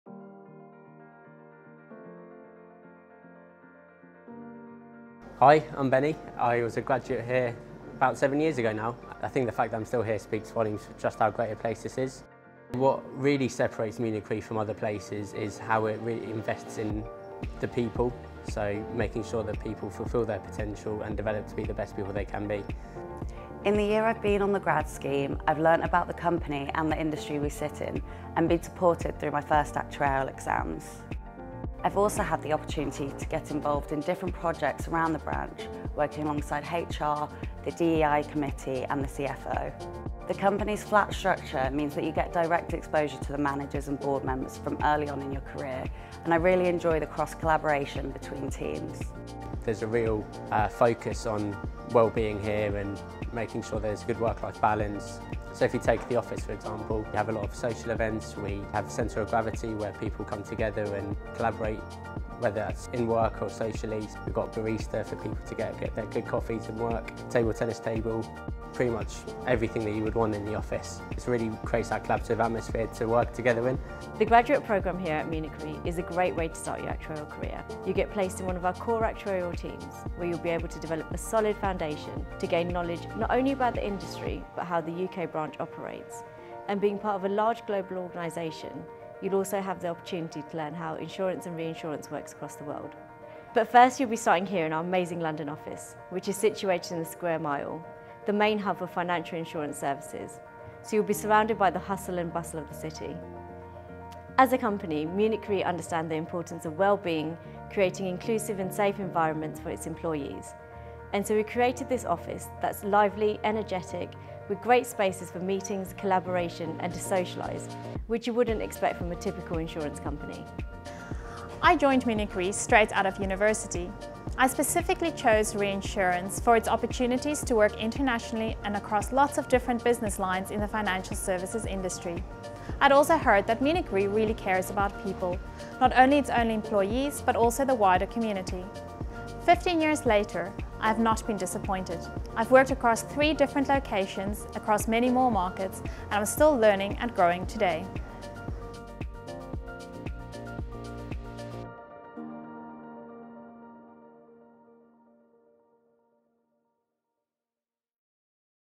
Actuarial Graduate Programme at Munich Re UK Life Branch
Unlock your full potential and kick-start your career at Munich Re, a leading reinsurance company. You will tackle real-world challenges in a supportive environment with a global network of experts at your fingertips.